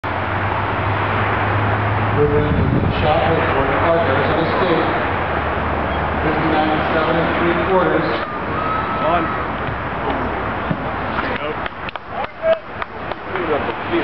Ryan Whiting 69' 9" at UCSD Triton Invitational 2010
Whiting's best throw at Triton invite